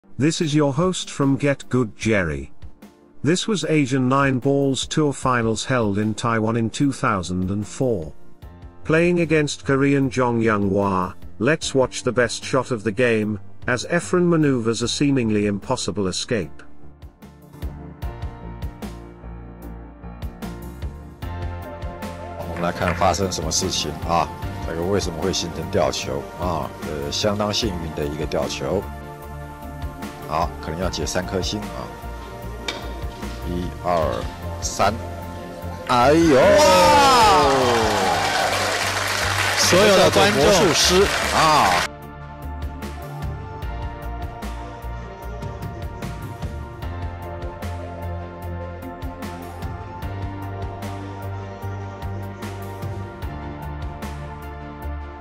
THE 4 RAILS KICK SHOT That Sends Migraine To His Opponent! Efren Bata Reyes | Get Good Gerry
GetGoodGerry